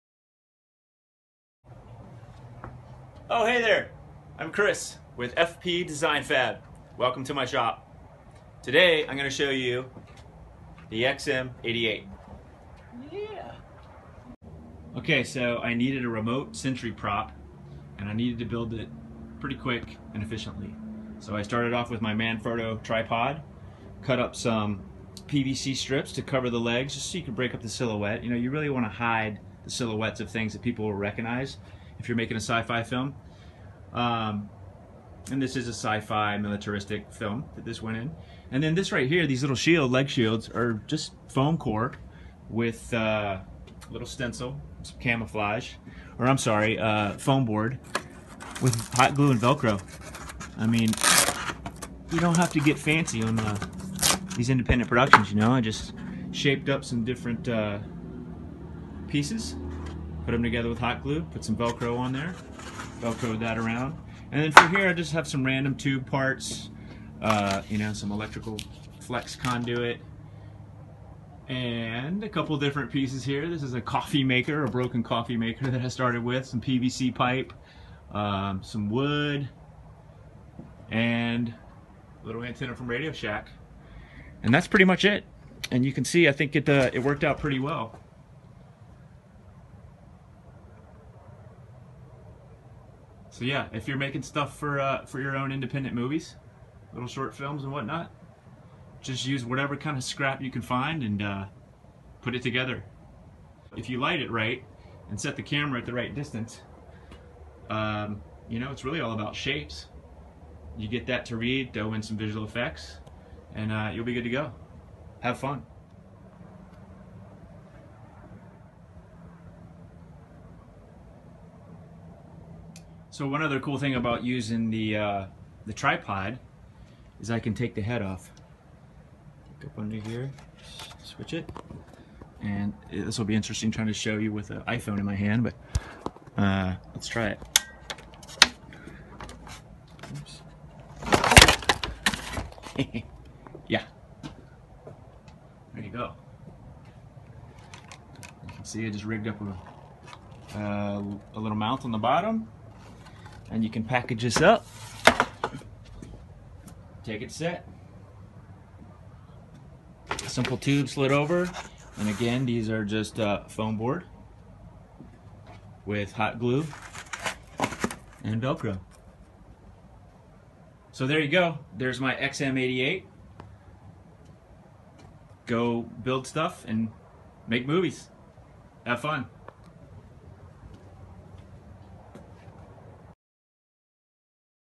XM-88 Sentry Gun Fast Film Prop
Design and fabrication of a Sci-fi remote sentry weapons prop for a short film. Here I give you a behind the scenes look at one of the film props I made in my shop. Tell a little about the simple design and fabrication to make stuff fast and efficiently for indie short films.